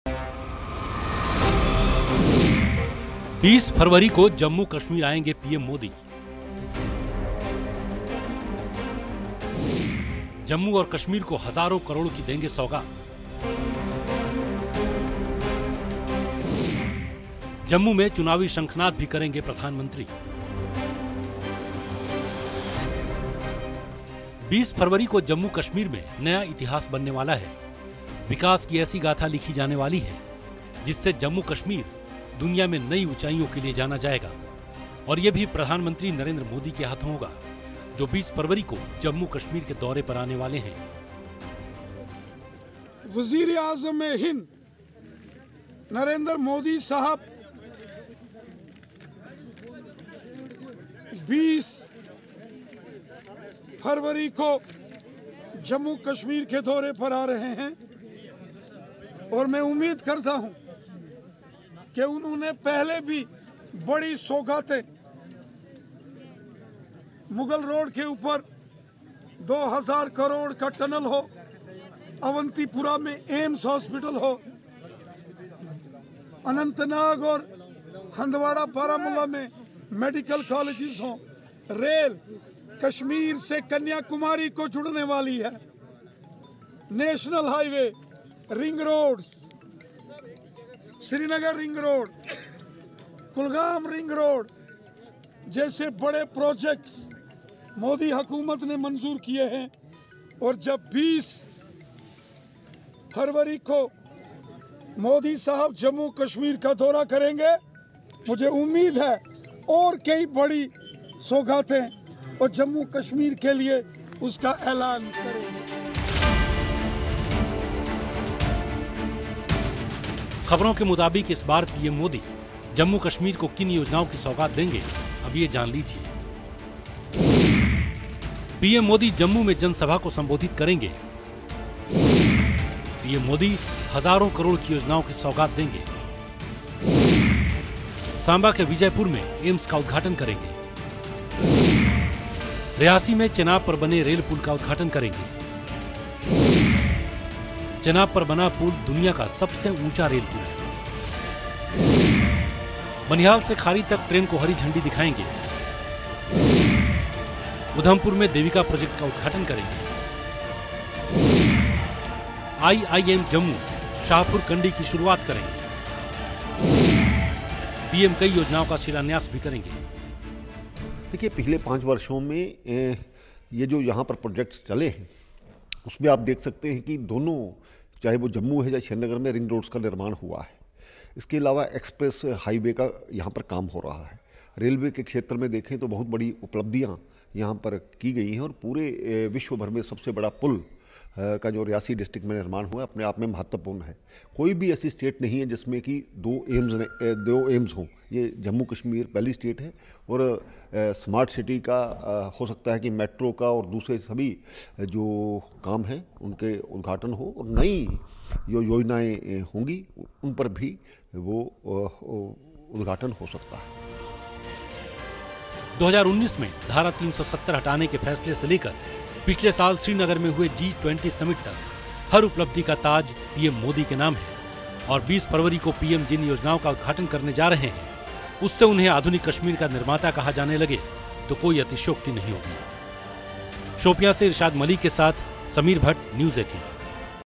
PM मोदी के जम्मू-कश्मीर दौरे में क्या कुछ रहने वाला है खास, जानिए
Special Report: PM Modi will visit Jammu and Kashmir on February 20, know what is going to be special in his visit.